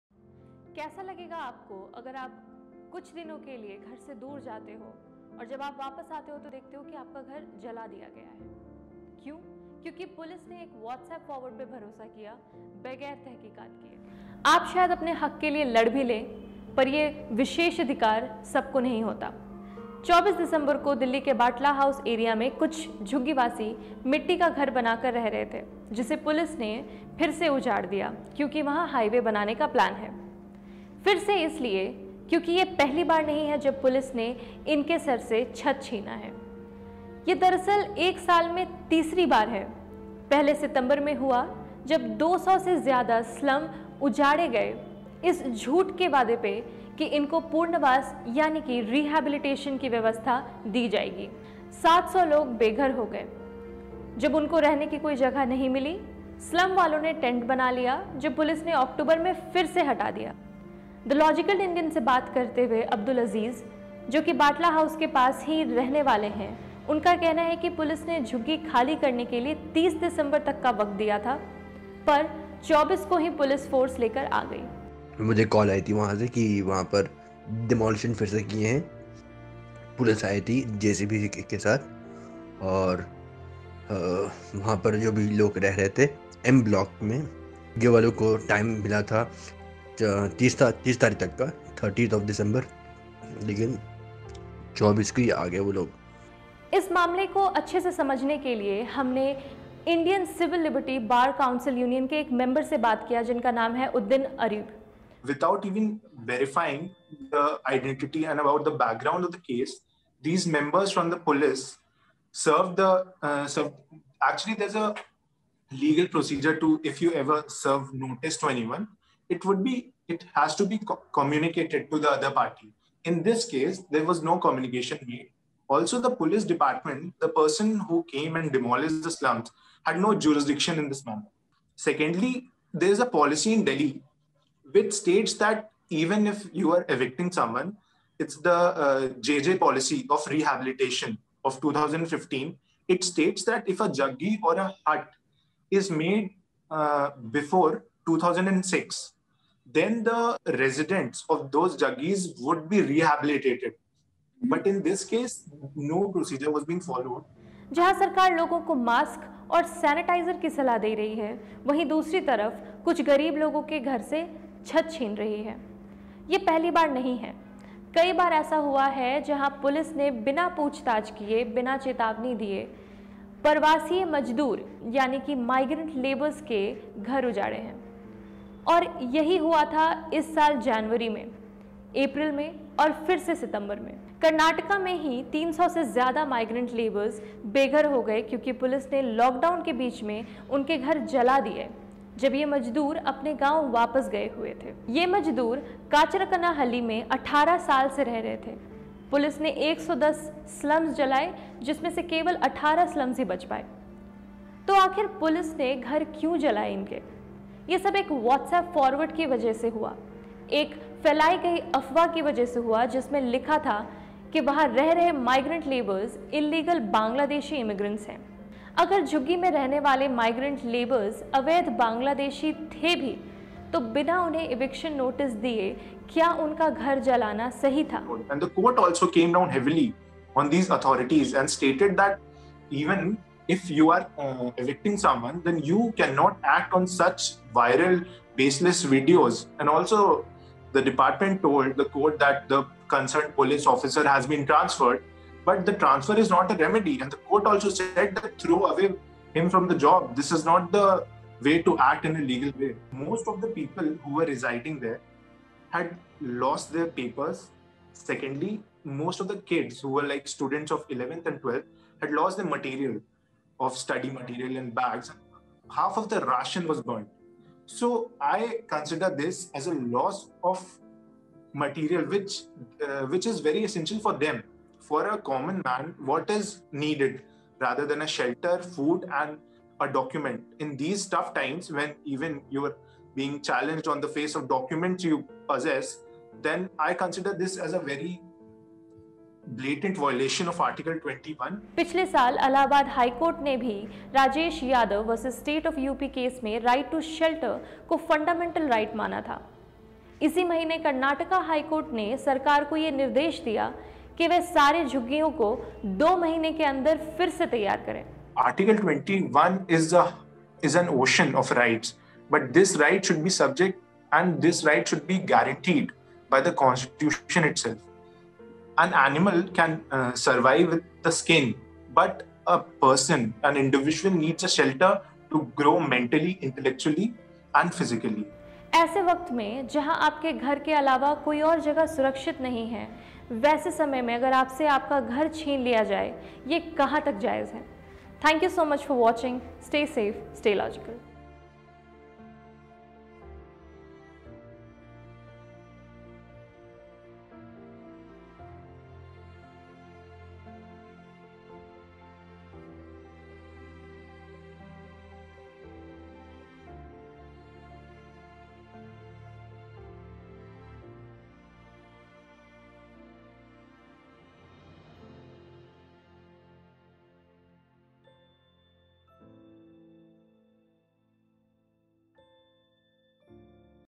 Right To Shelter: Served In Karnataka, Denied In Delhi. Batla Slums Destroyed Again.
The Logical Indian’s Anukriti Ganesh tells us about Right to Shelter and how it was served in Karnataka this month while Delhi slums are getting destroyed over and over again.